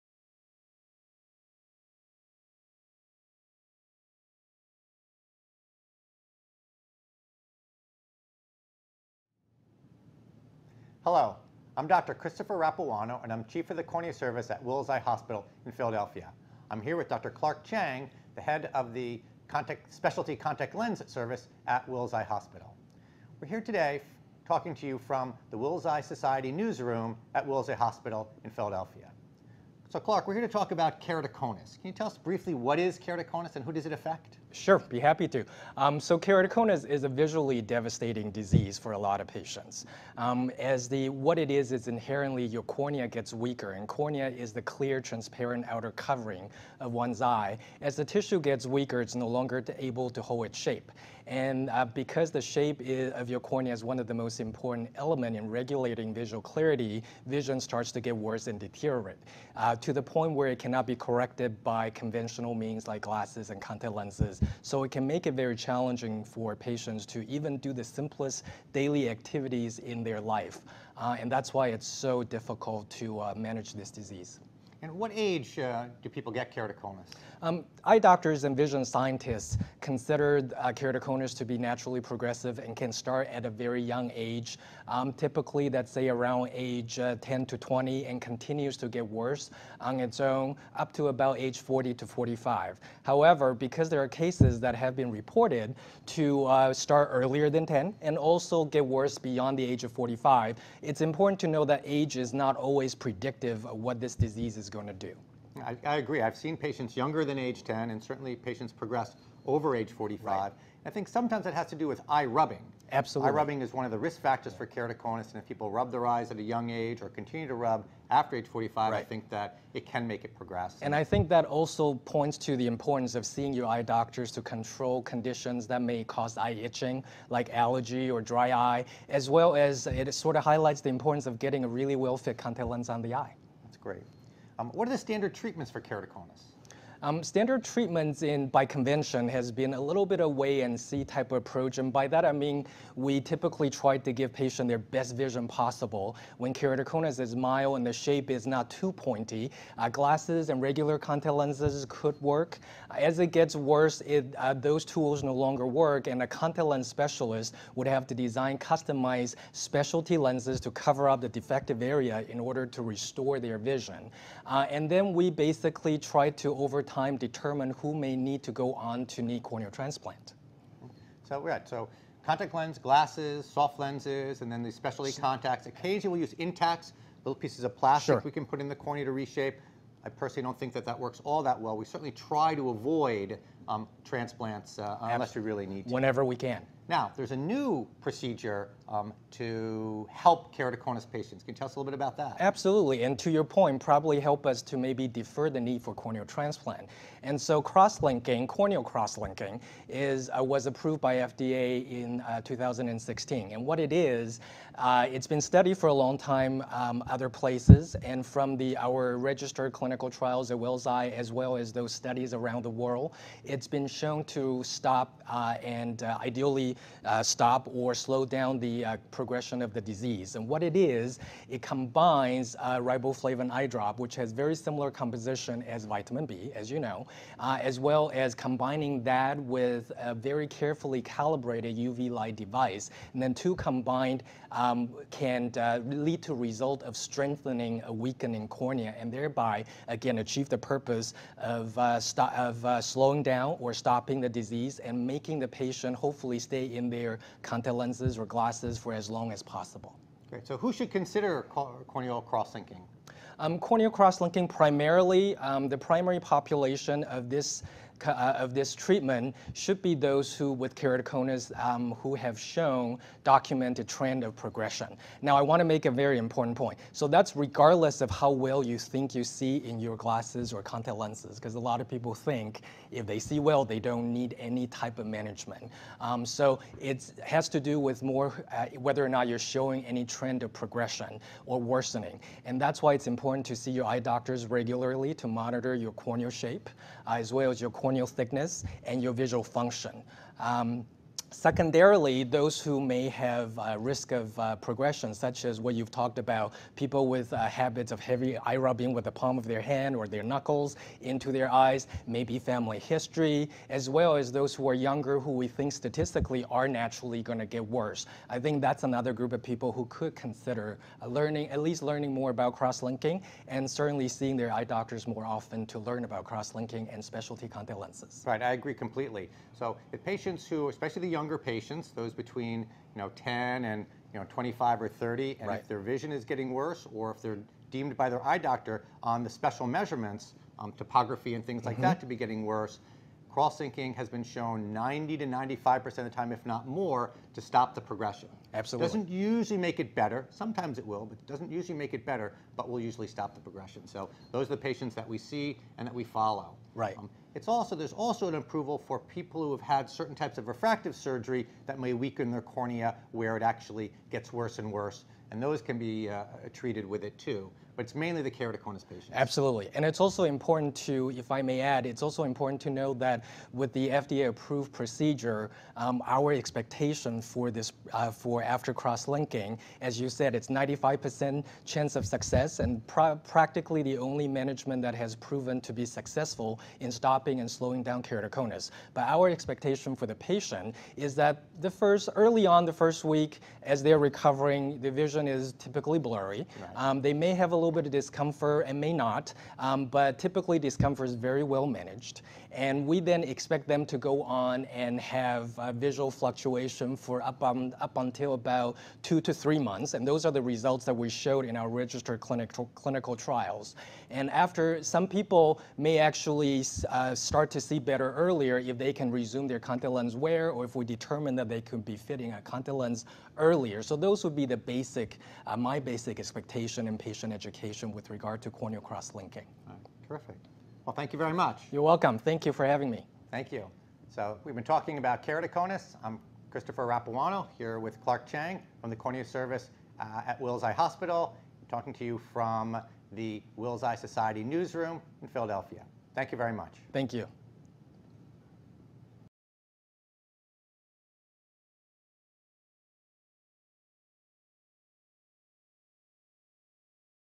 Keratoconus Treatments: Corneal Crosslinking & Specialty Contact Lenses
Keratoconus Treatments: Corneal Crosslinking & Specialty Contact Lenses is discussed by Christopher J. Rapuano, MD- Chief
 and Clark Chang, OD, MSA, MSc, FAAO of the Wills Eye Hospital Cornea Service